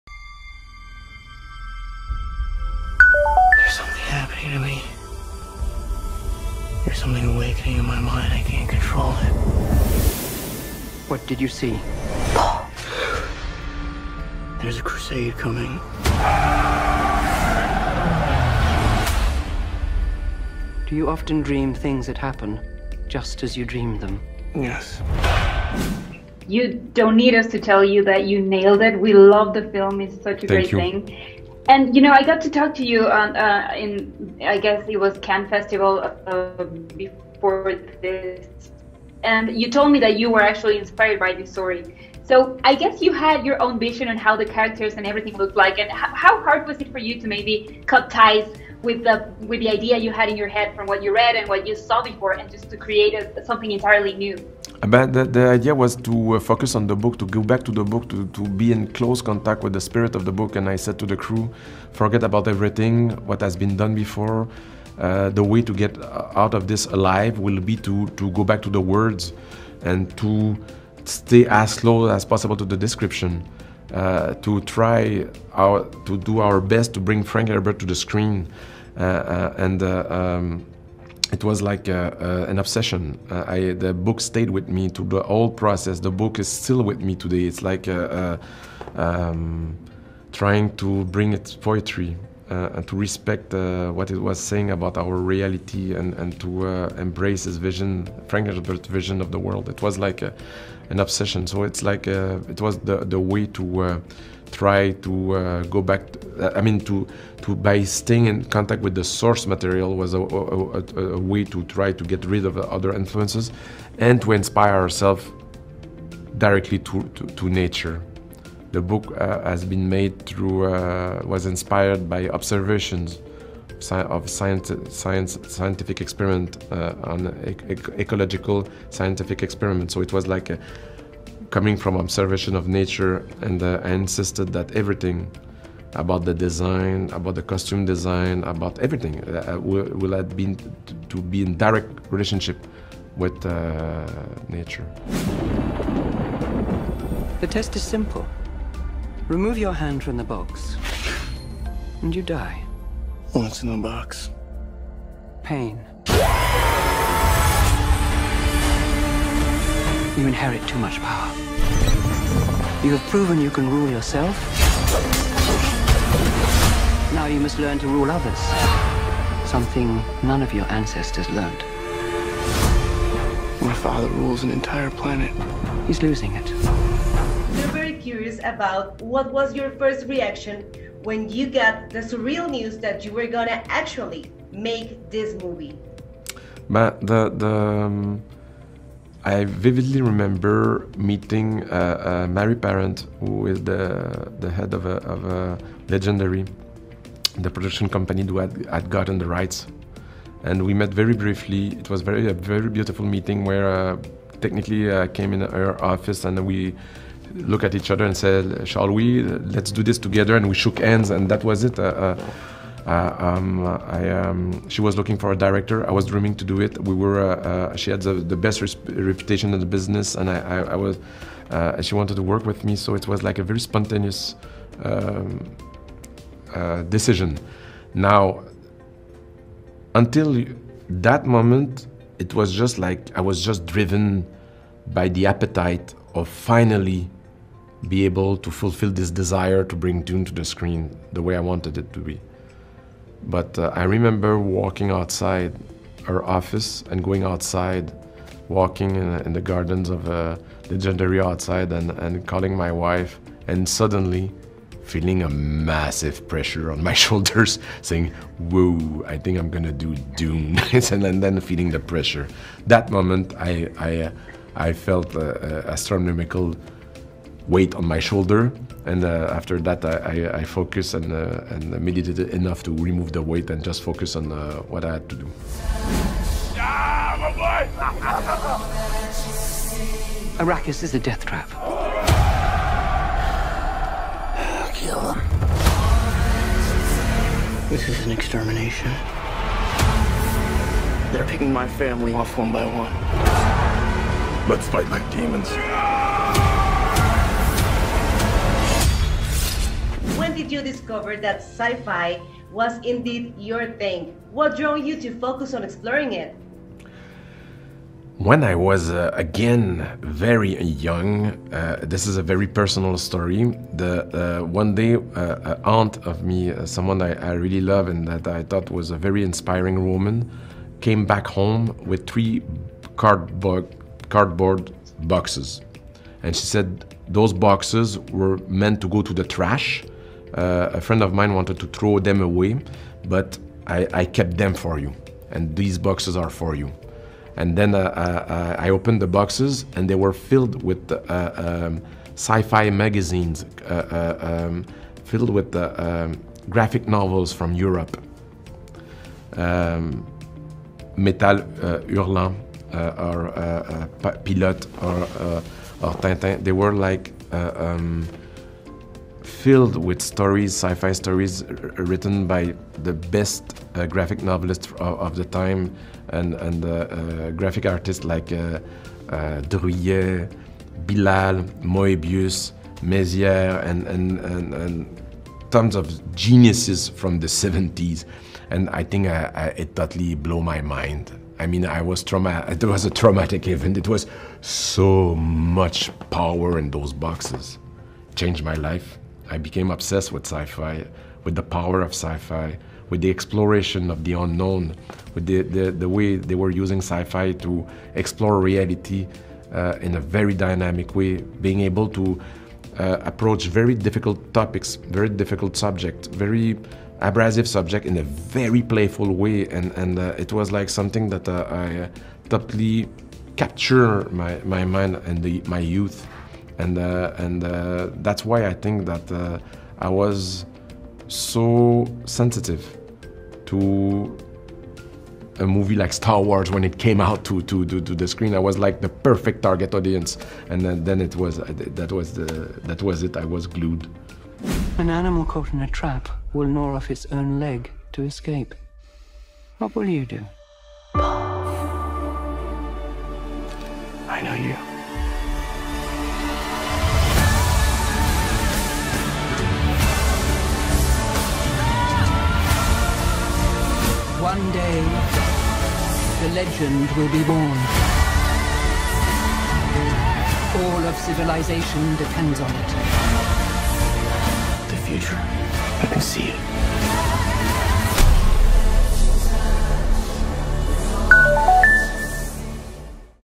DUNA: DENIS VILLENEUVE dirige cast de ensueño | Entrevista
Gaby Cam 40 y Riva PopMusic platicaron con el director canadiense acerca de cómo llegó a este proyecto, su reacción al saber que sería parte y en qué momento surgió su amor por el cine.